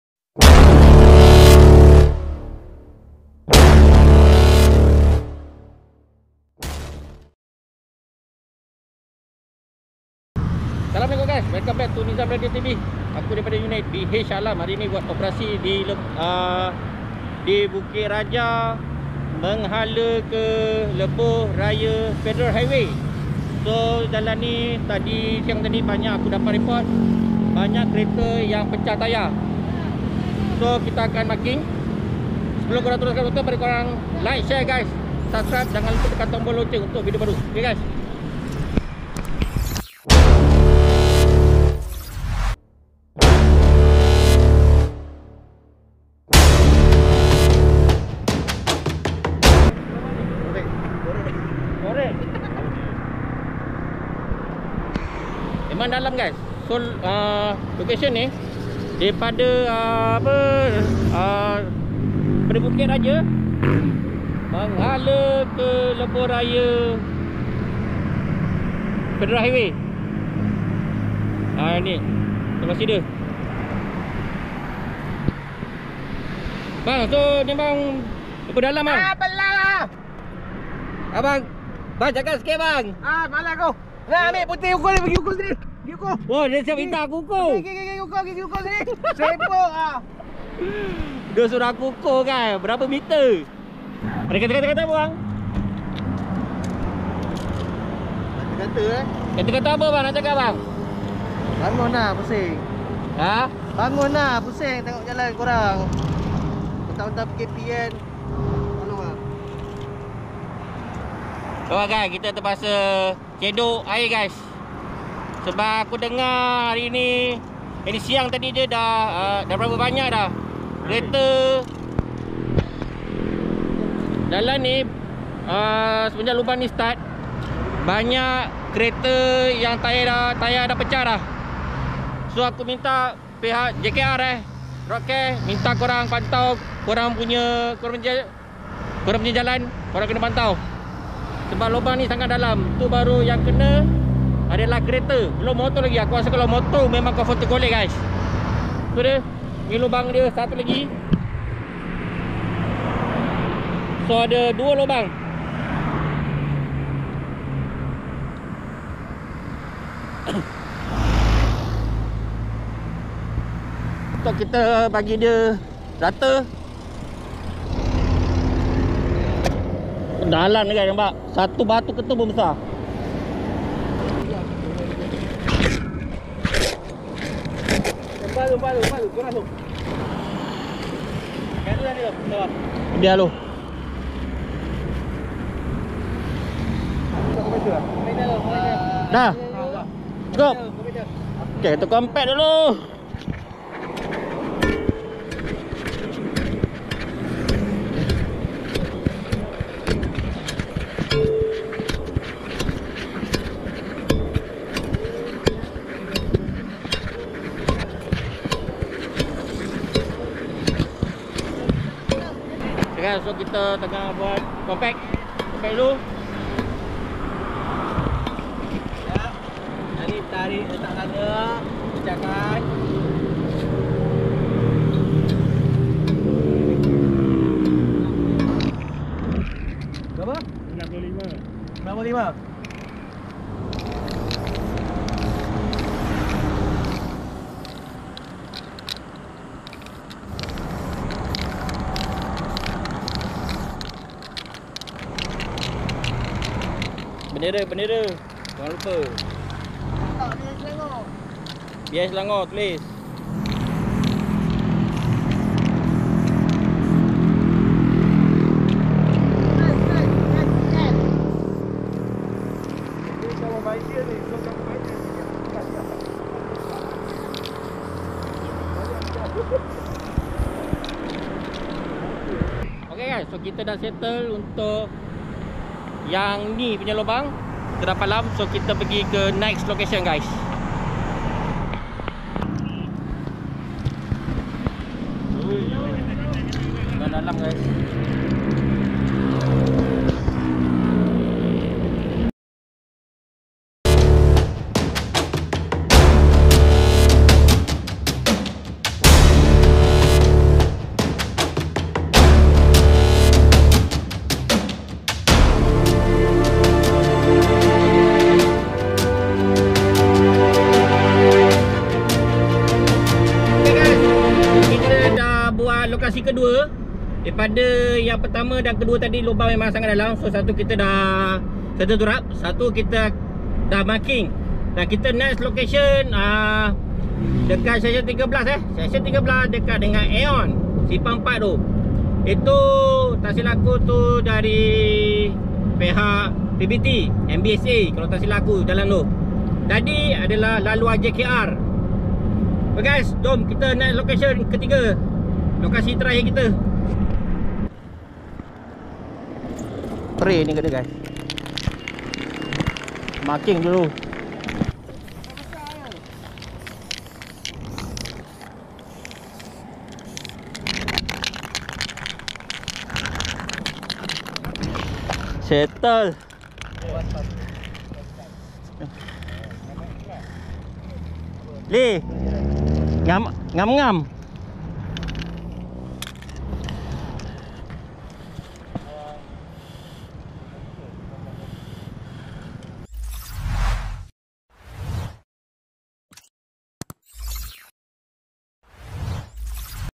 Brotherhood Selangor Turap Lubang Maut
28/9/2021

Brotherhood selangor telah membuat operasi di 3 lokasi malam tadi

lokasi

1. Dari bukit raja susur masuk faderal highway

2. Dari i-city susur masuk faderal highway

3. Simpang empat aeon seksyen 13 shah alam, persiaran akuatik

Mohon pihak berwajib jalankan tugas anda.